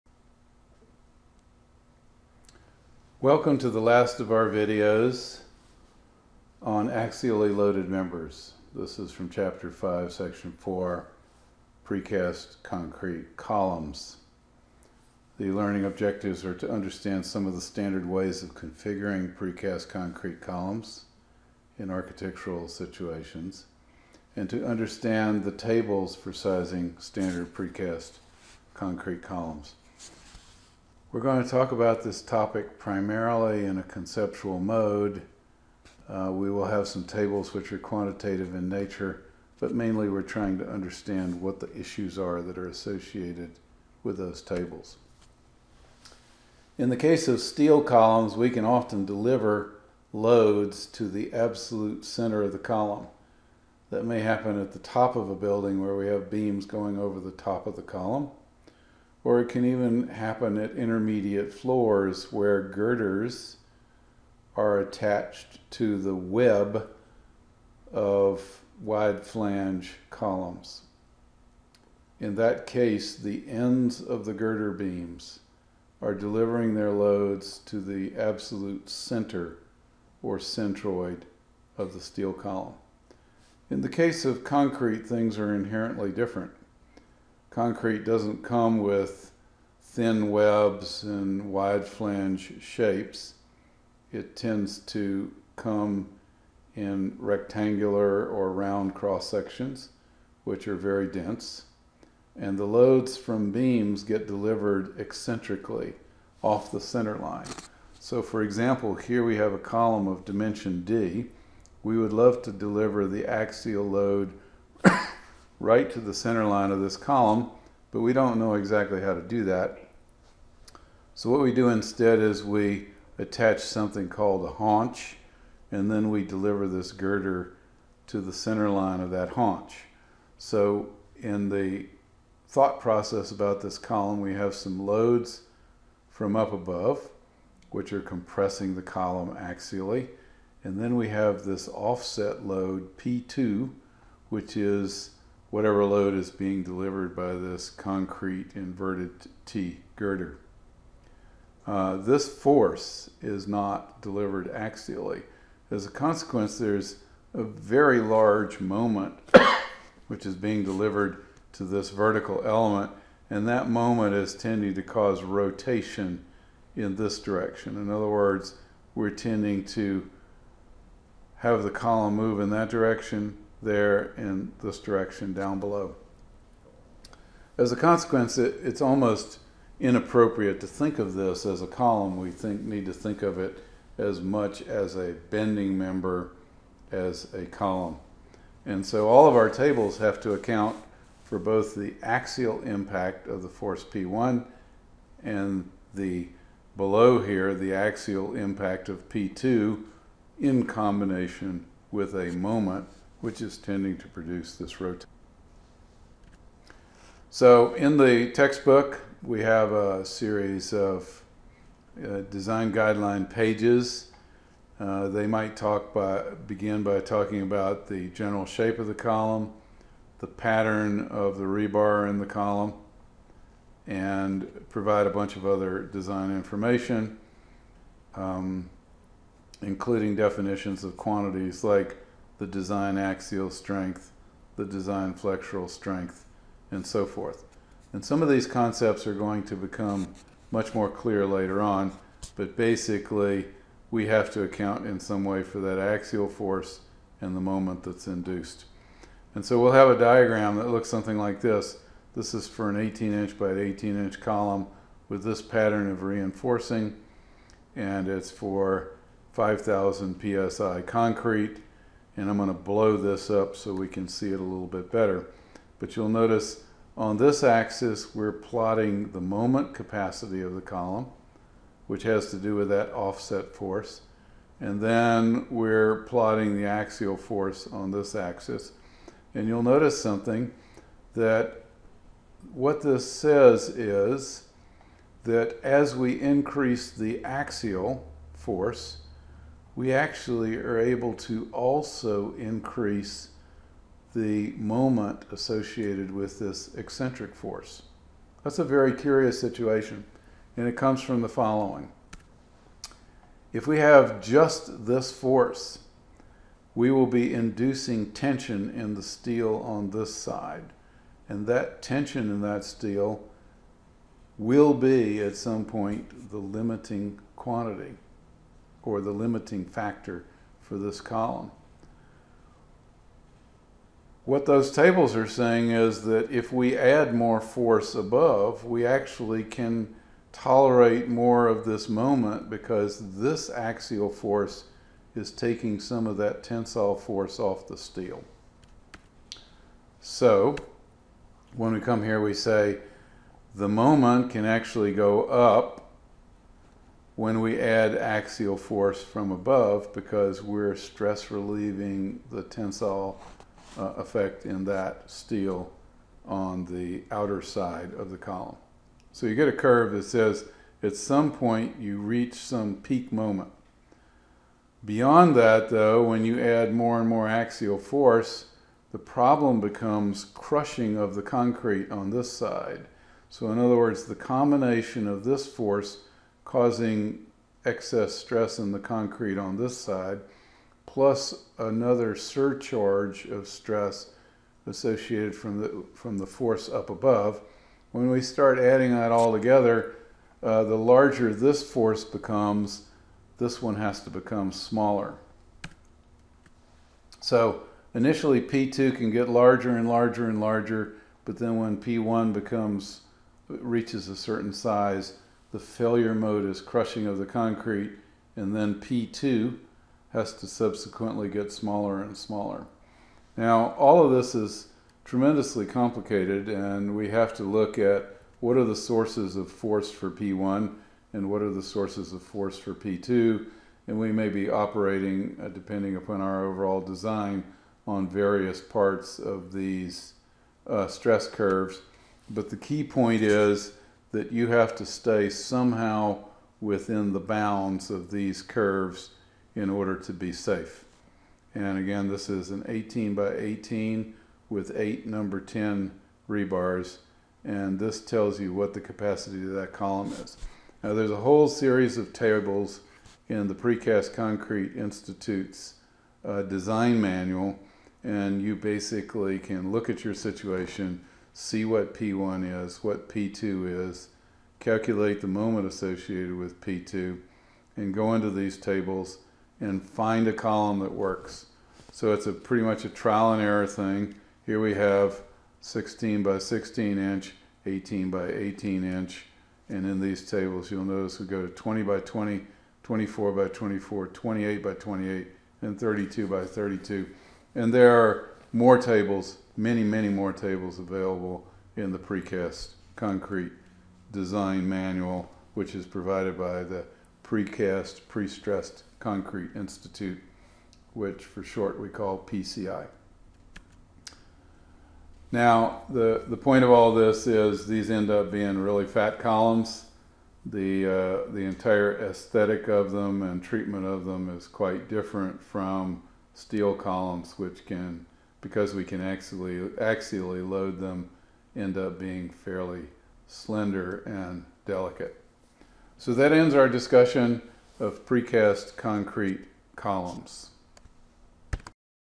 05 4 Precast Concrete Columns
NC State University College of Design, School of Architecture, courses ARC 331 and ARC 332, Architectural Structures taught by Wayne Place, Professor of Architecture, PhD, PE